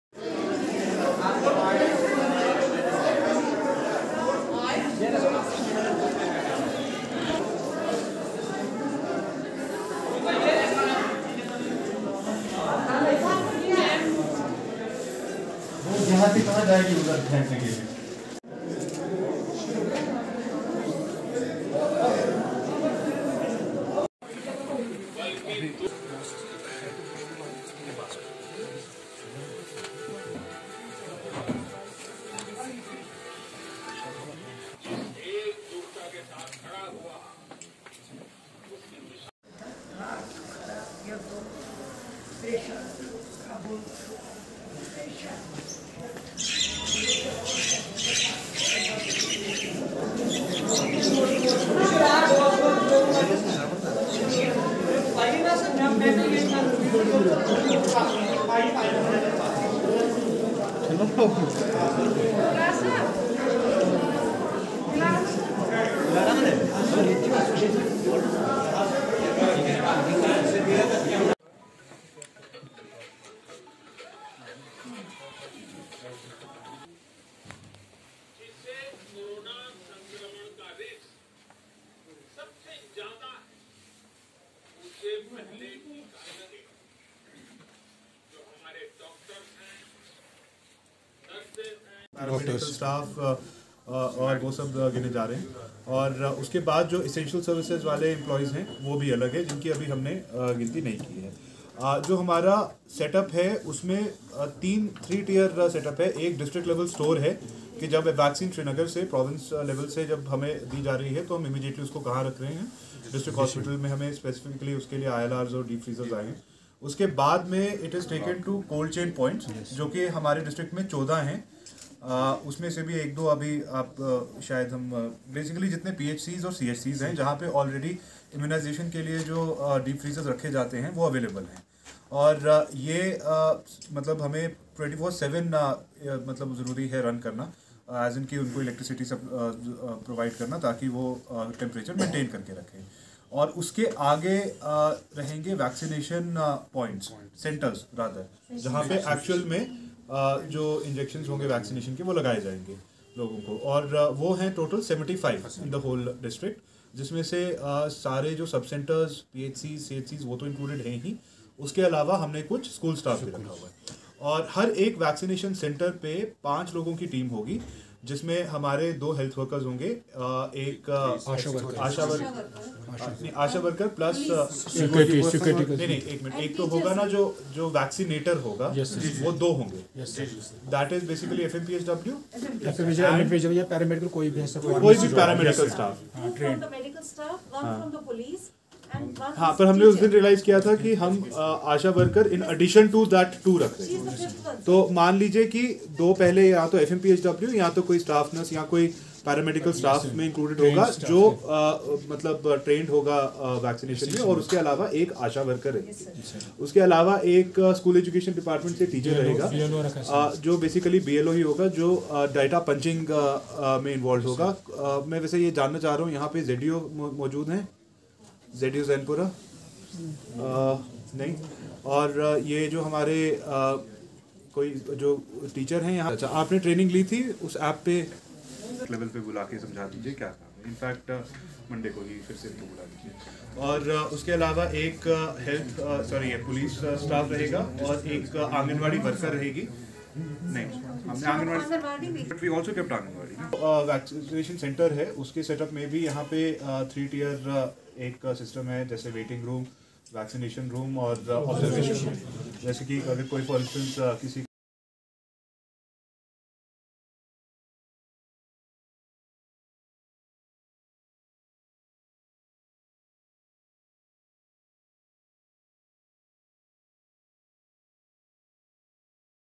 Vaccination drive started at Zainapora by DDC Shopian Yaseen Choudhary
Vaccination drive started at Zainapora by DDC Shopian Yaseen Choudhary in presence of other officials

Around one hundred workers to get shots today in Zainapora